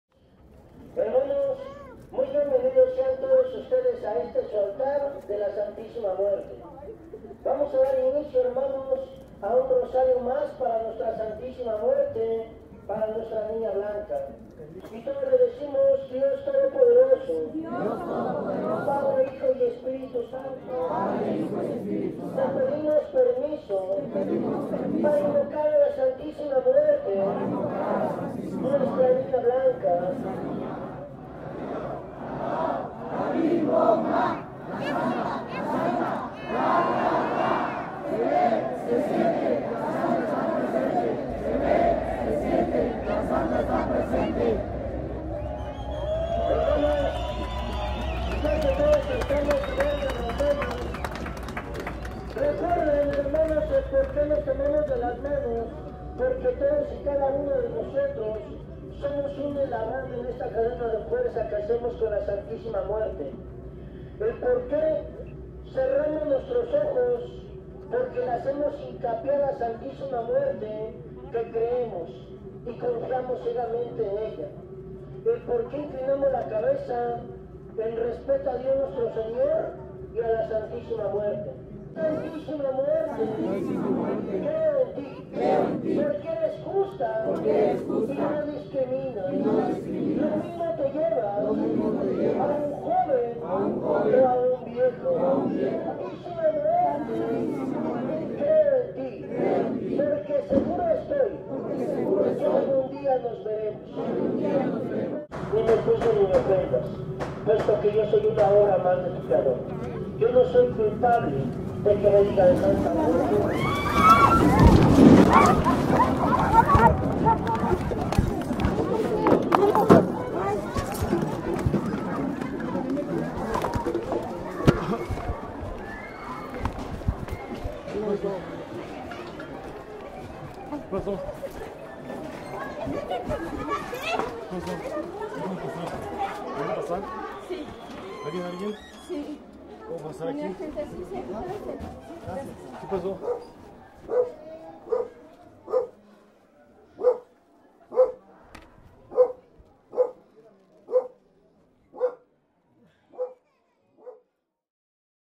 Culto a la Santa Muerte
Días de celebraciones y rezos, de rendir culto y tributo. De hacer ofrendas y altares dedicados a la protagonista de éstos días, a la Señora, a la dueña de los rezos de los devotos asistentes. Se trata de la Niña Blanca, también conocida como la Santa Muerte. Son 3 días de celebrar, de rendir culto, de mandas y ofrendas, de montar los altares y de vestir a la Santa… (Continúa leyendo